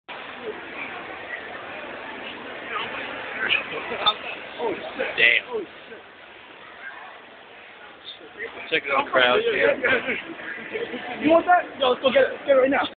Black Friday Madness
Me in a Circuit City at 5AM, Black Friday.